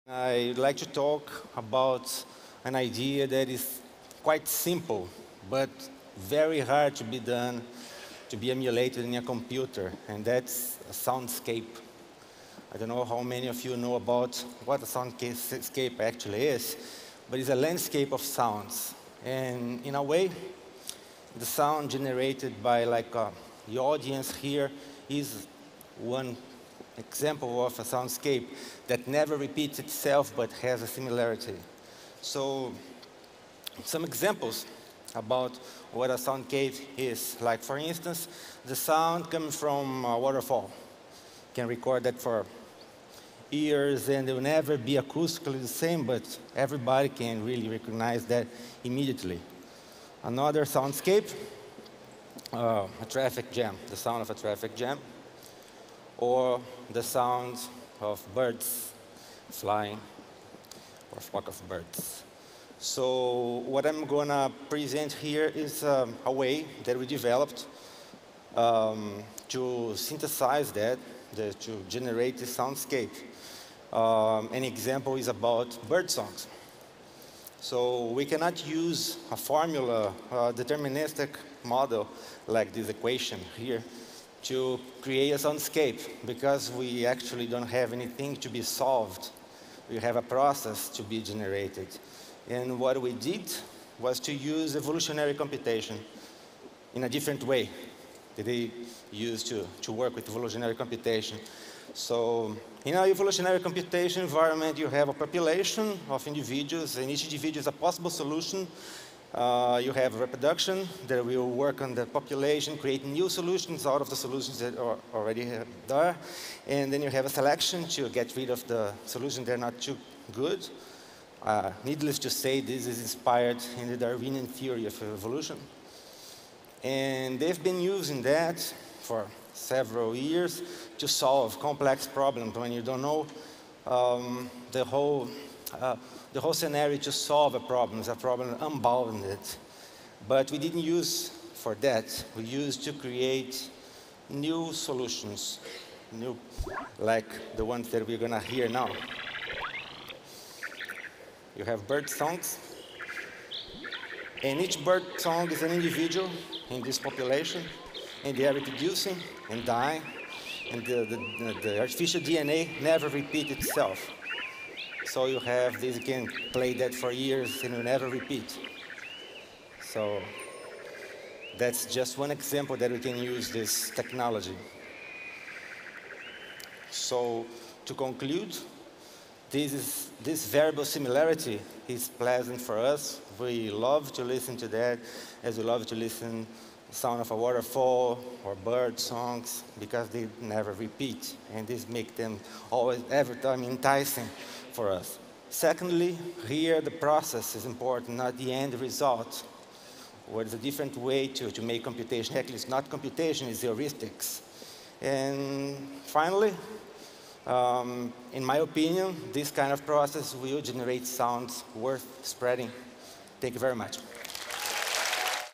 Jose Fornari: An algorithm to mimic nature's birdsongs
Pianist and techie Jose Fornari works at the Interdisciplinary Nucleus for Sound Communication at the University of Campinas, where he creates original soundscapes from birdsong.